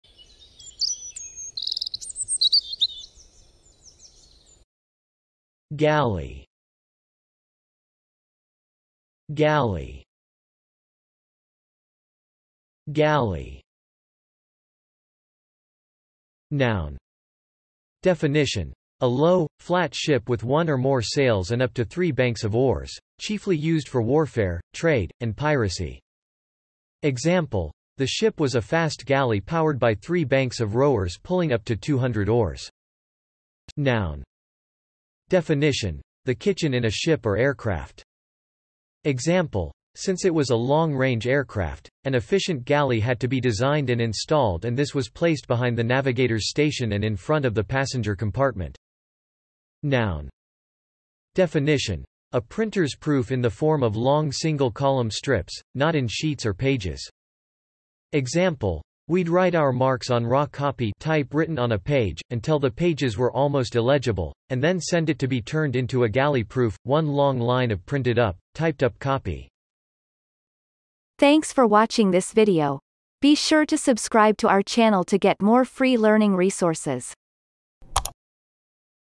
How to pronounce galley - Vocab Today
The pronunciation of the word galley in Amercian accent is demonstrated in this video. If the word is from another language, such as brand name, it will be pronounced in its original language. Based on the word frequency, this is of 3 stars difficulty.

The word galley has the following meanings:
1. (noun)
Definition : A low, flat ship with one or more sails and up to three banks of oars, chiefly used for warfare, trade, and piracy.
Example : The ship was a fast galley powered by three banks of rowers pulling up to 200 oars.
2. (noun)
Definition : The kitchen in a ship or aircraft.
Example : Since it was a long-range aircraft, an efficient galley had to be designed and installed and this was placed behind the navigator's station and in front of the passenger compartment.
3. (noun)
Definition : A printer's proof in the form of long single-column strips, not in sheets or pages.
Example : We'd write our marks on raw copy (type written on a page) until the pages were almost illegible, and then send it to be turned into a galley proof (one long line of printed up, typed up copy).